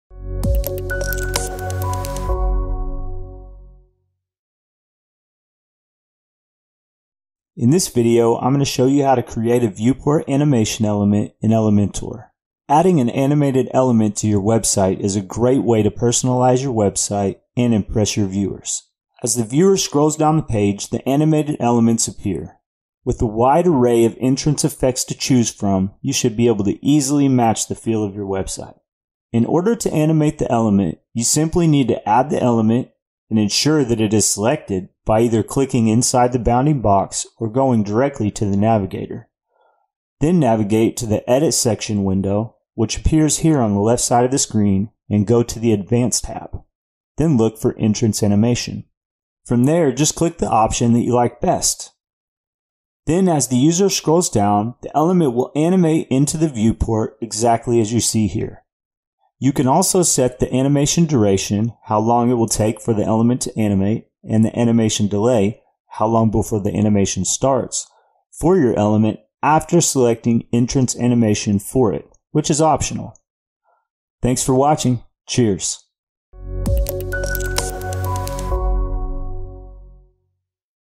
How to create a viewport animation element in Jupiter X
In this Artbees video tutorial, Chris shows how to create an animation effect for an element entrance using Elementor page builder.